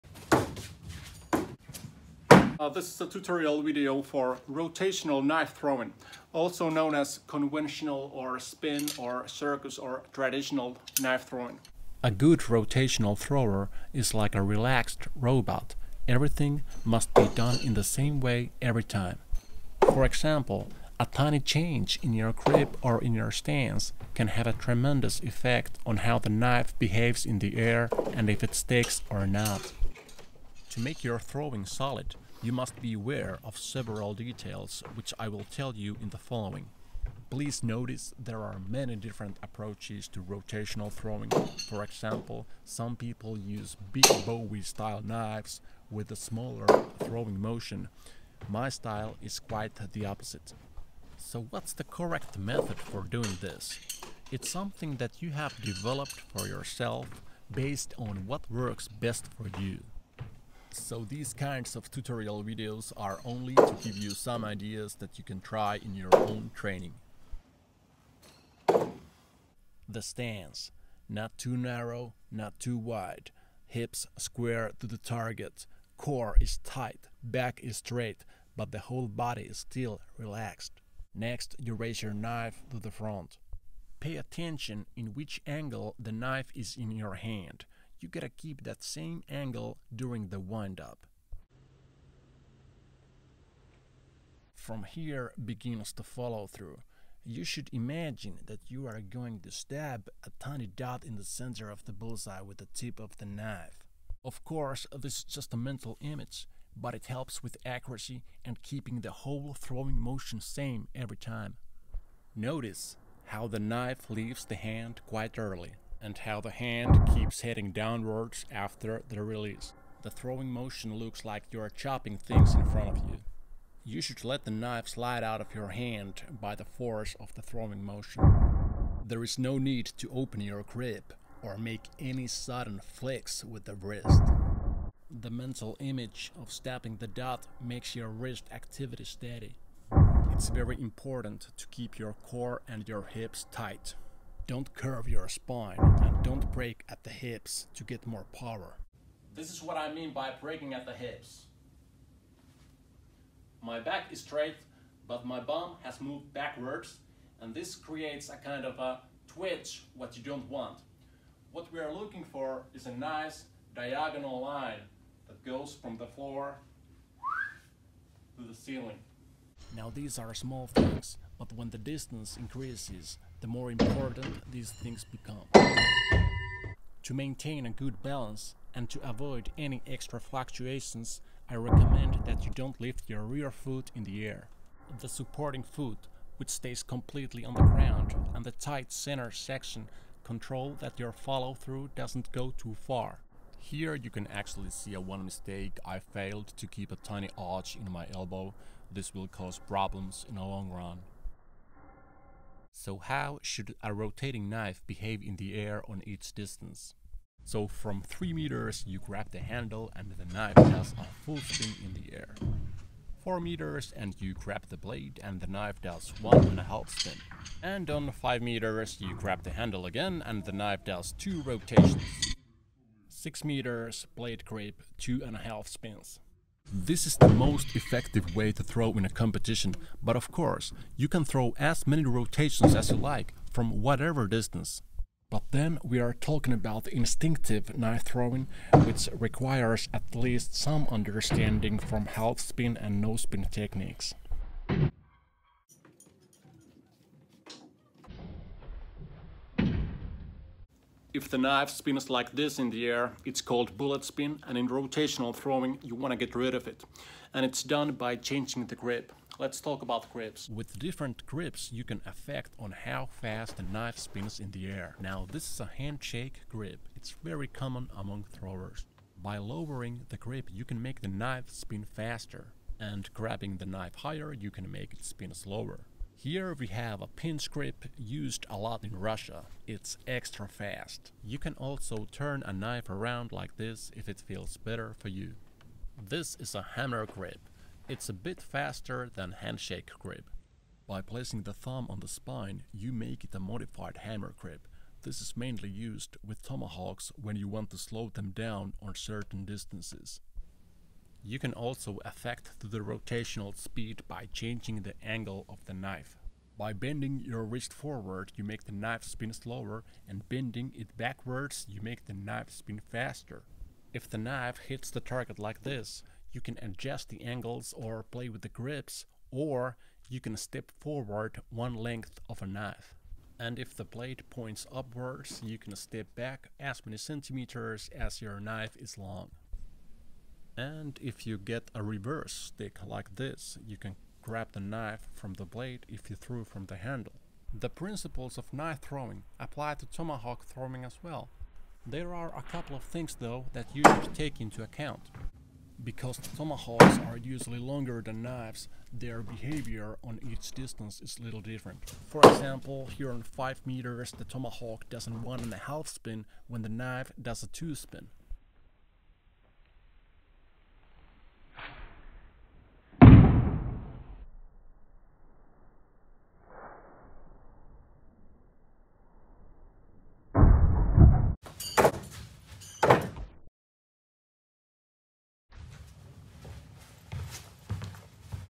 Rotational knife and axe throwing tutorial
The nordic friends 🇸🇪🇫🇮 offer you a double tutorial on rotational knife throwing!

For the first time, The Swedish pro thrower Marcus Pehart (aka Soulthower.com) shares his teachings in a very detailed way.

In addition, Joel Holopainen, mainly known from no spin circles, gives a little different approach to rotational throwing. Marcus uses bigger knives with smaller throwing motion (American style) whereas Joel uses smaller knives with larger body movement (Russian style).

No comprehensive tutorials on rotational throwing has been made in english before. Maybe because people tend to think that rotational throwing is quite simple but it’s actually full of little details which can be honed for a lifetime to attain full mastery.